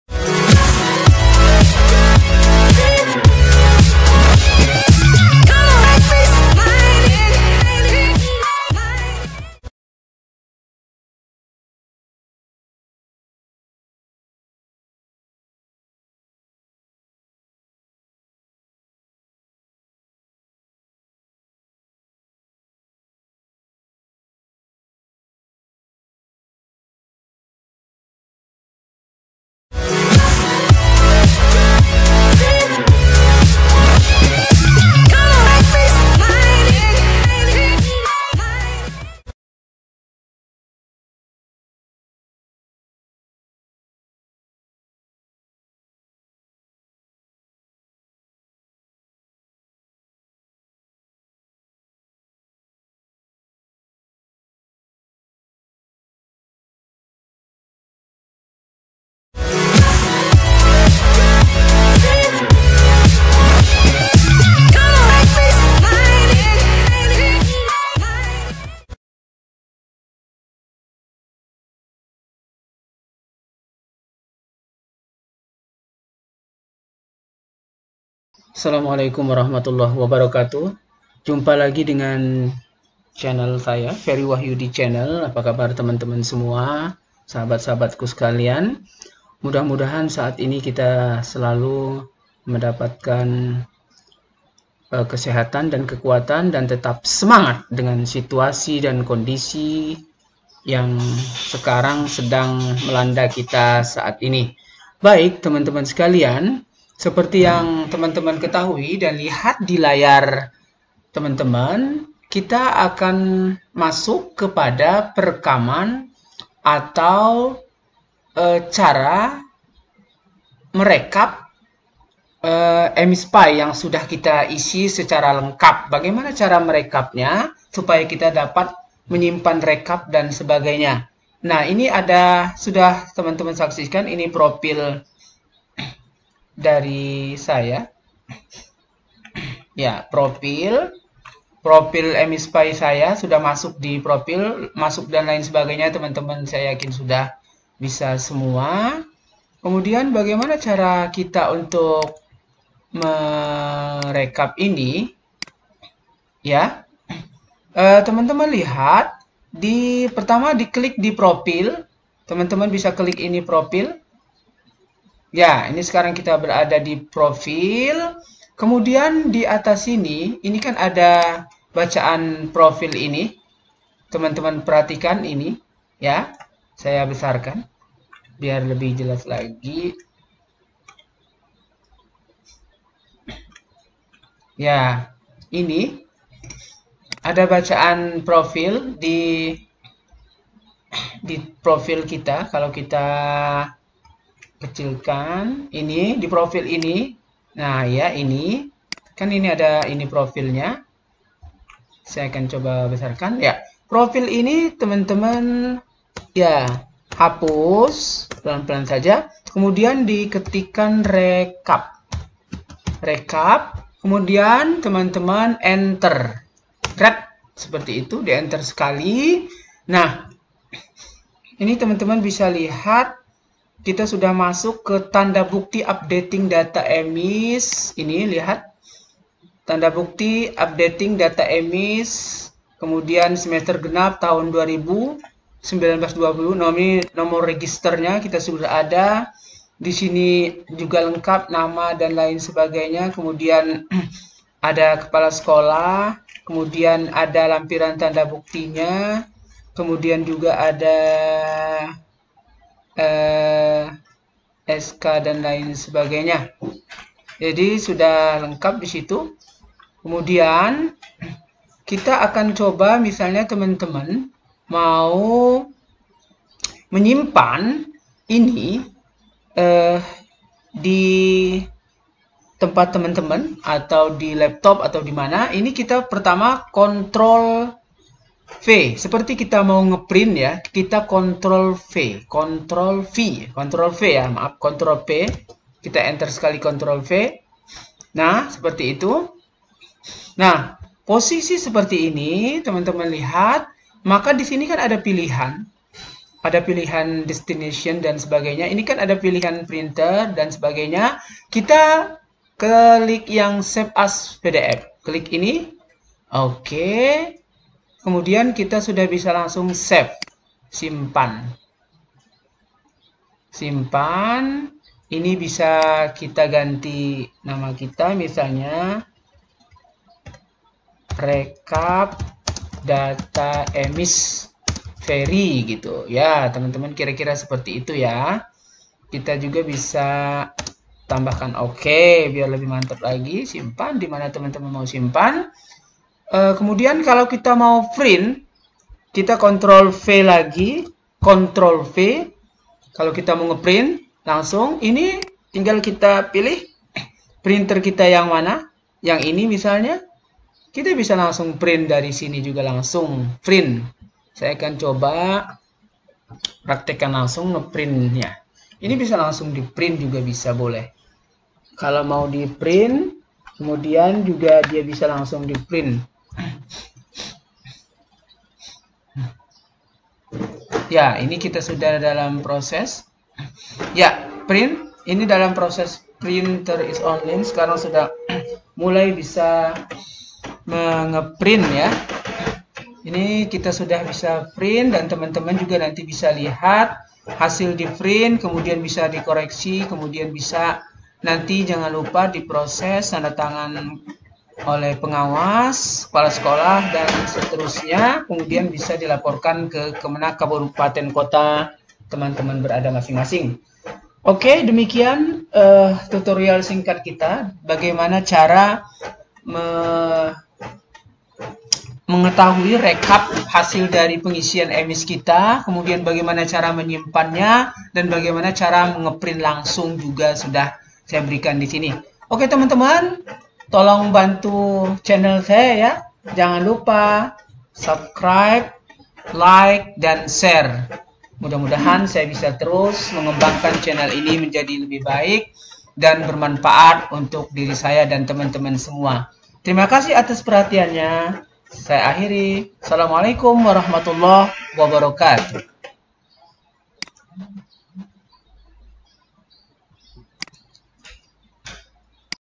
cara rekap emis pai 2020 dari profil
ini adalah video tutorial tentang cara mengetahui hasil rekap dari data emis pai yang telah kita isi melalui profil.
face book : feri wahyudi